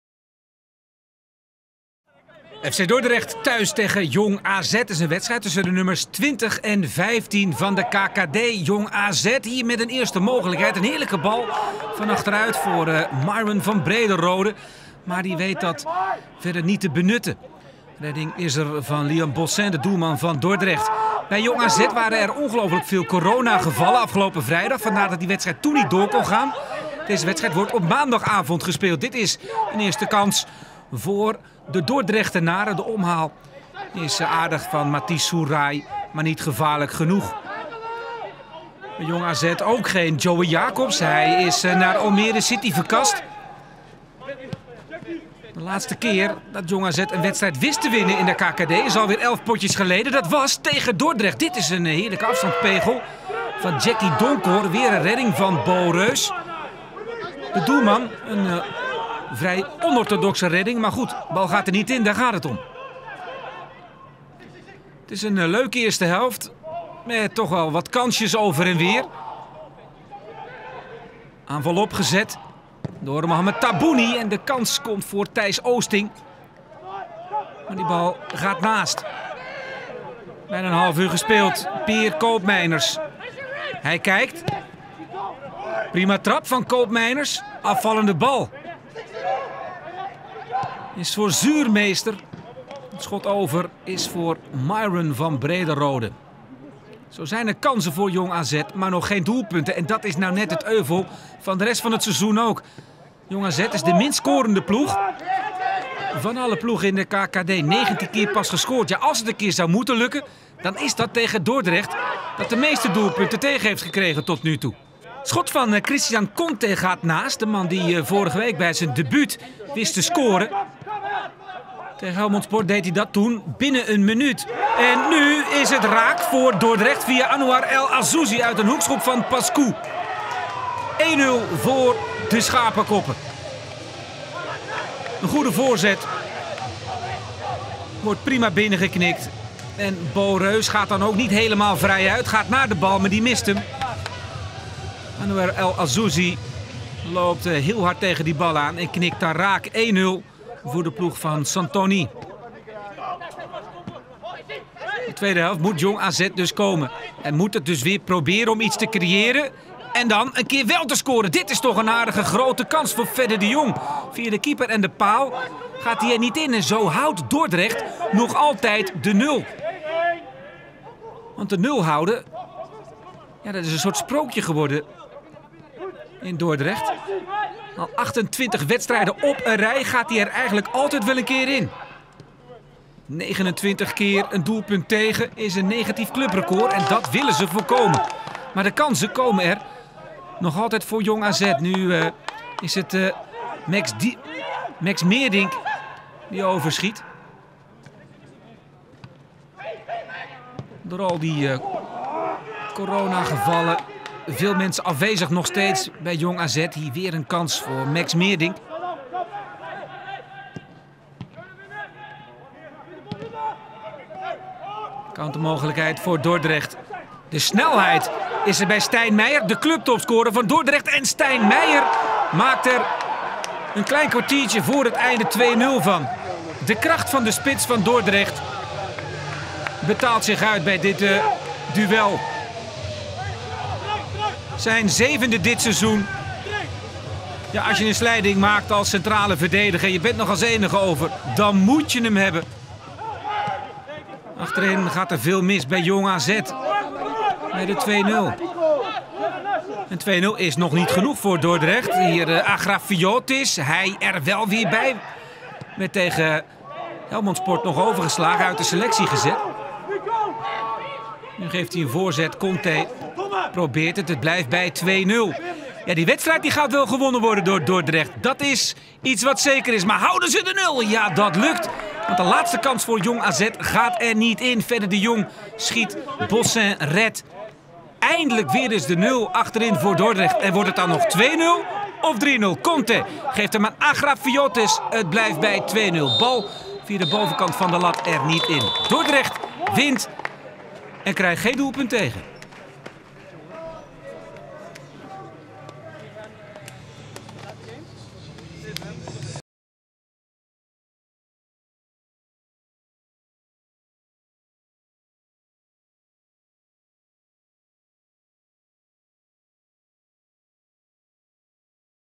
Highlights FC Dordrecht - Jong AZ | Keuken Kampioen Divisie
Samenvatting van het Keuken Kampioen Divisie-duel tussen FC Dordrecht en Jong AZ op het Riwal Hoogwerkers Stadion.

Scoreverloop: 33. El Azzouzi 1-0, 79. Meijer 2-0.

Opstelling FC Dordrecht: Liam Bossin; Ruggero Mannes, Toine van Huizen, Anouar El Azzouzi, Mauro Savastano; Mathis Suray (86. Alessio Miceli), Ersin Zehir (81. Seydine N'Diaye), Pascu (75. Emir Biberoğlu), Jacky Donkor (75. Jerailly Wielzen); Christian Conteh, Stijn Meijer (81. Nikolas Agrafiotis).

Opstelling Jong AZ: Beau Reus; Jorn Berkhout, Peer Koopmeiners (64. Maxim Dekker), Sem Dirks, Richard Sedláček; Fedde de Jong, Mohamed Taabouni, Zico Buurmeester (64. Lewis Schouten); Ernest Poku (64. Iman Griffith), Thijs Oosting (64. Mexx Meerdink), Myron van Brederode (75. Soulyman Allouch).